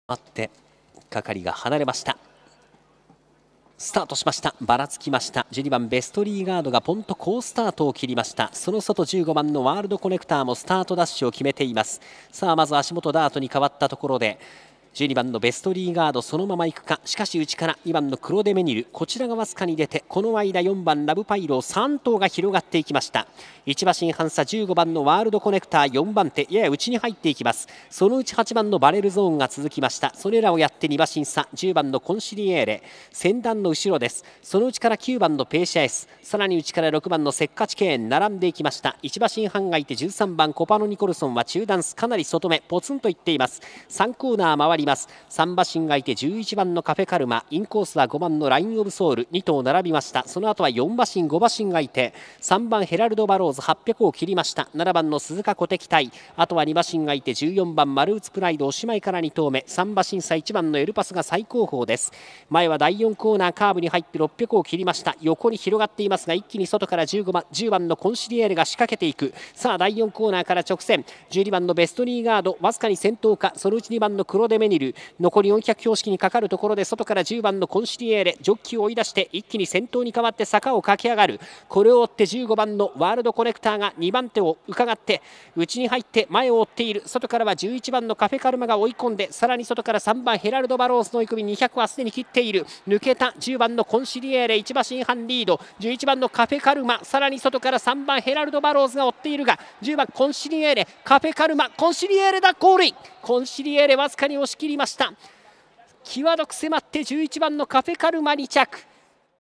2021.11.27 Cattleya Stakes (JPN) - Consigliere
2yo, Dirt 1600 Meters Open, Japan Road to the Kentucky Derby
1.Consigliere (Drefong - Tasha's Star, by Spanish Steps) Christophe Lemaire - Yukio Inagaki 1:38.0
2.Cafe Karma
3.Geraldo Barows
4.World Connector
6.Copano Nicholson 8.Marubutsu Pride 11.Best Regard